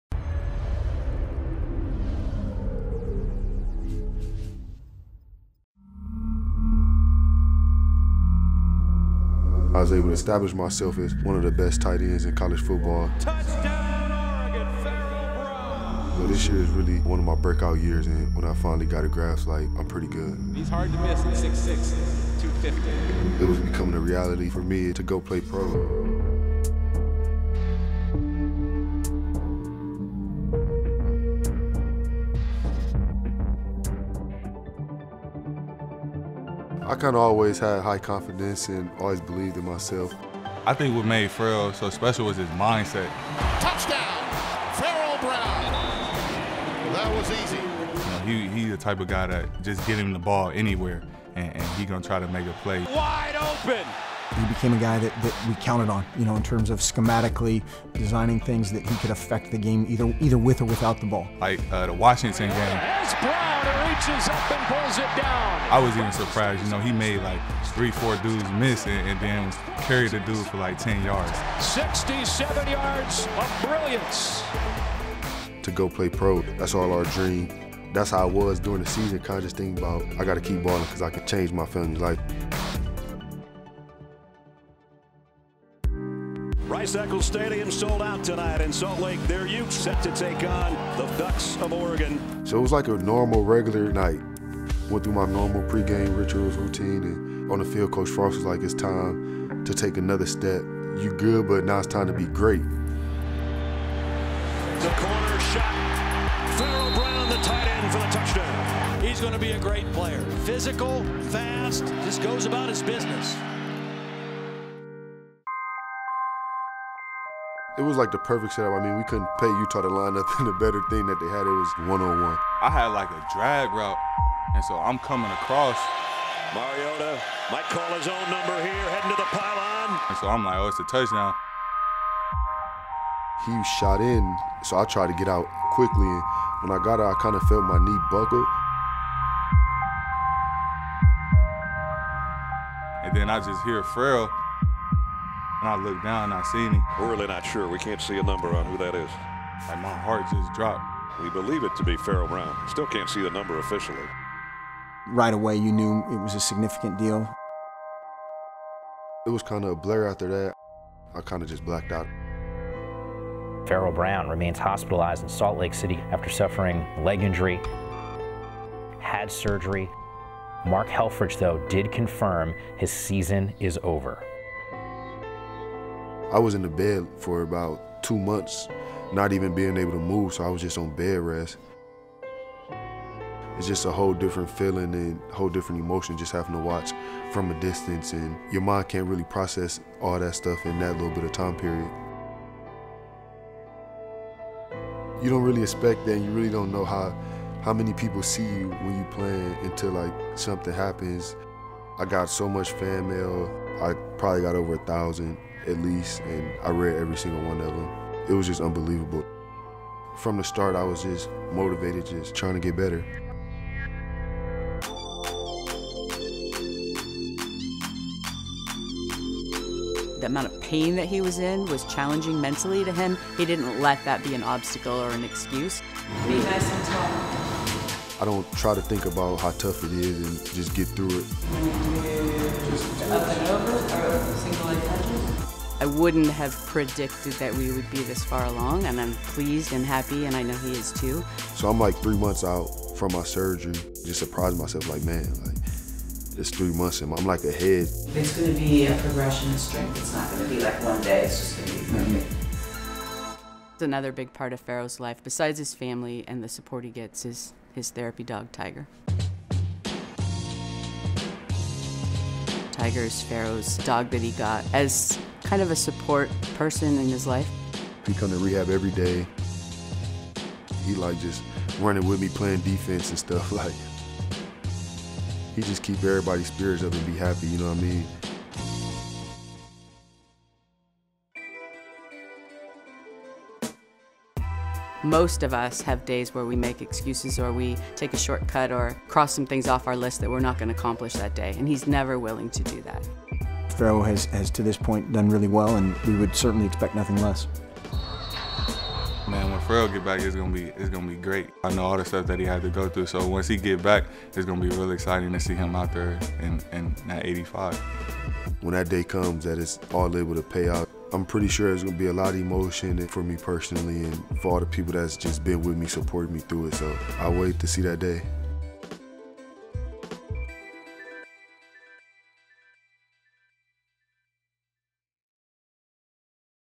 Oregon's Pharaoh Brown surpassing expectations to overcome dangerous leg injury
Oregon tight end Pharaoh Brown talks about the recovery he's currently going through after a severe leg injury in a game back on Nov. 8 required him to undergo surgery and stay bedridden for two months. See how he's now overcoming the medical obstacles to get back on the field, including a little help from his friend Tiger.